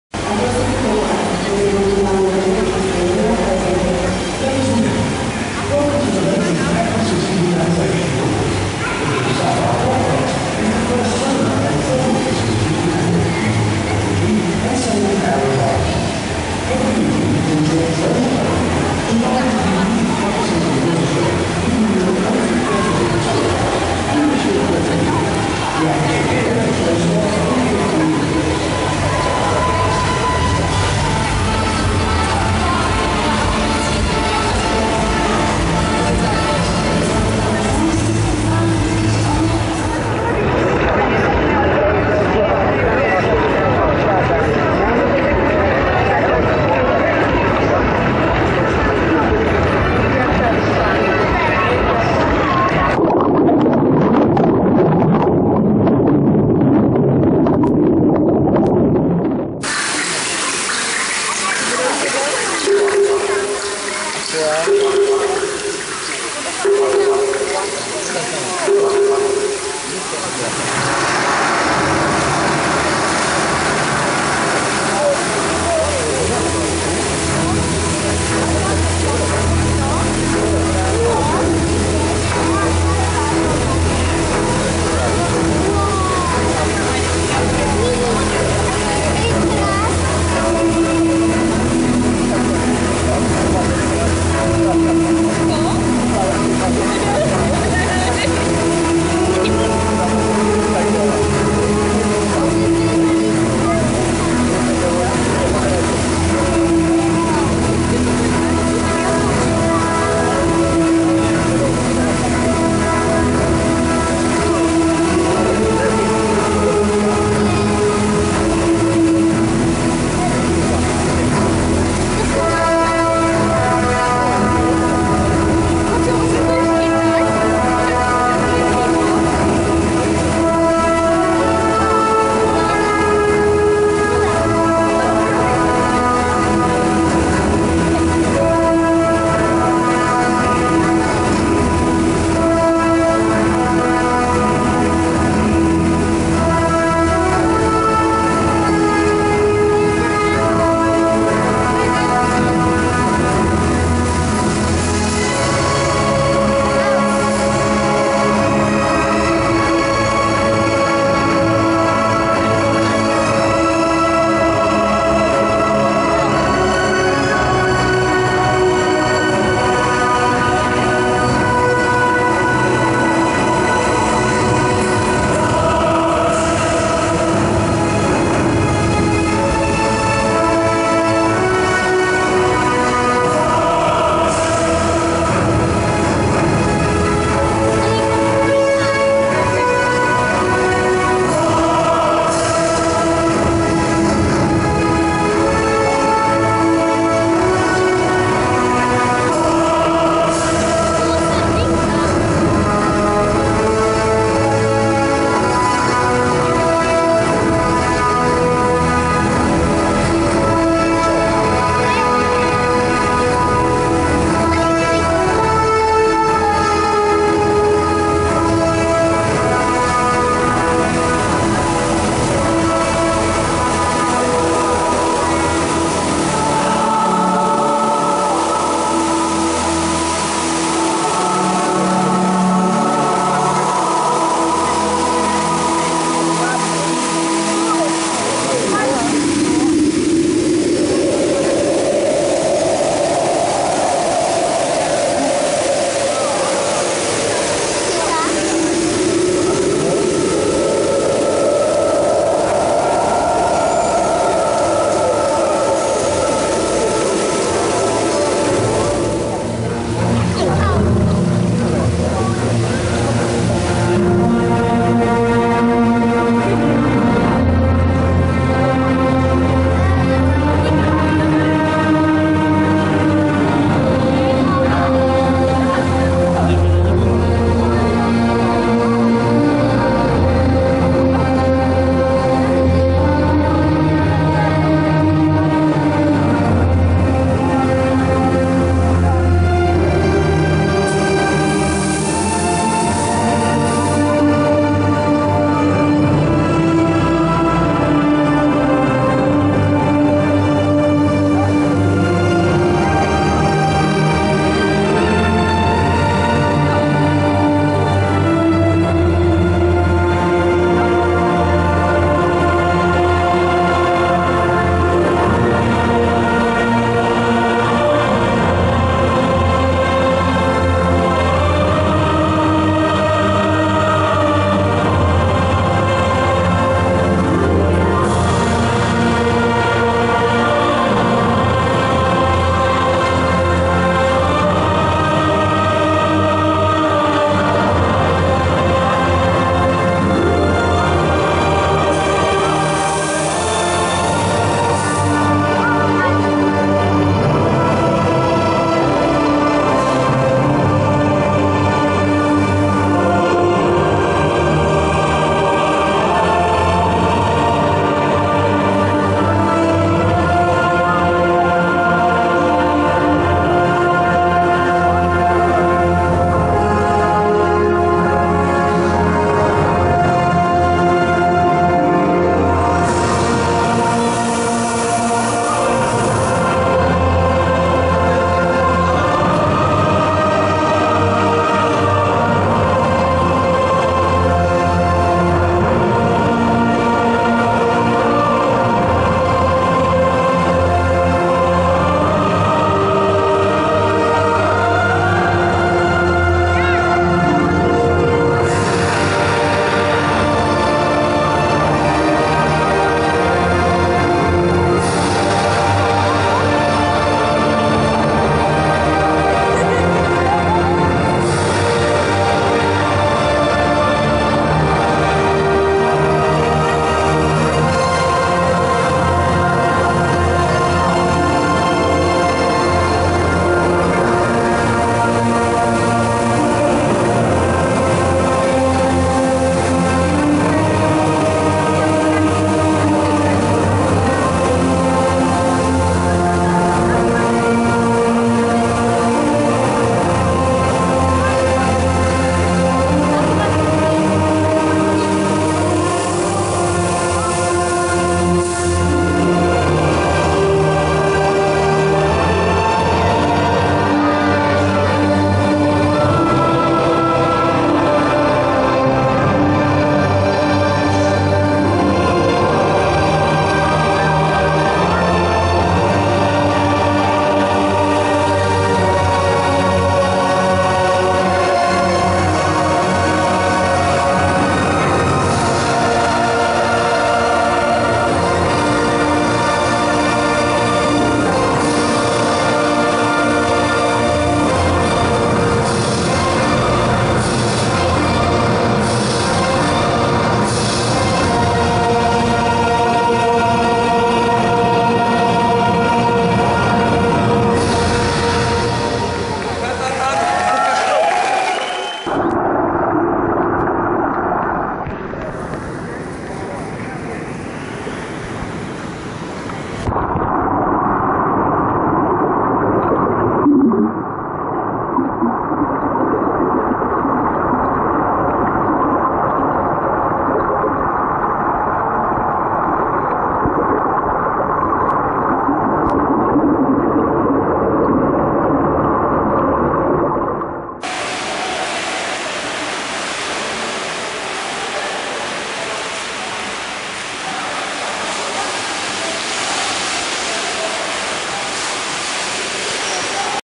аквапарк AquaCity Poprad Slovakia
лазарное шоу в аквапарке г.ПОПРАД Словакия

автор видео не решает сколько рекламных роликов должно быть в видео